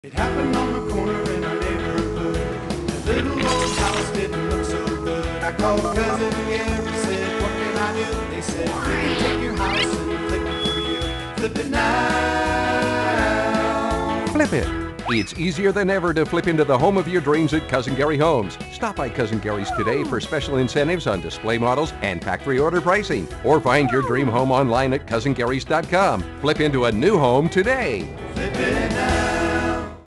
Cousin Gary Homes Flip It! TV ad 07/08 (surf it!)
Cousin Gary Homes' summer fun Flip It TV ad. Music by Rob Steen. Art by Mike Wiebe. Voice over by Jack Barry. Edit by Chris Westbay of Skycrest Media. Cousin Gary Homes is a manufactured home dealer serving Northern California (from the Sacramento Valley to the Oregon border) and specializing in affordable, turn-key housing solutions which compare with site-built quality and features.